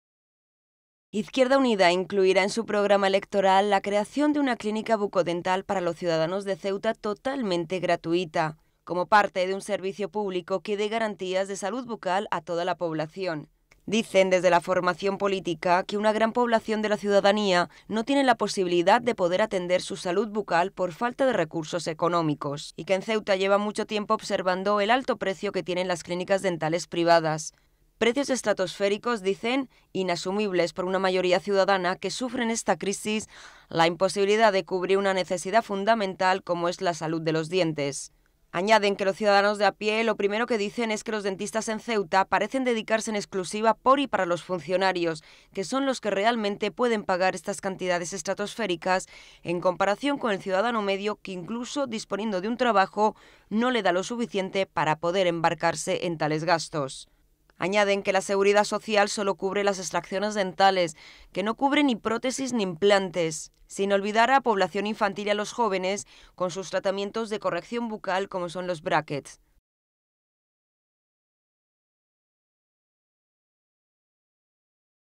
IU apuesta por una clínica bucodental gratuita para los ciudadanos
La formación política IU incluirá en su programa electoral la creación de una clínica bucodental gratuita para los caballas. Dicen, que en Ceuta "llevan observando desde hace mucho tiempo el alto precio que tienen las clínicas dentales privadas, precios estratosféricos e inasumibles por una mayoría ciudadana".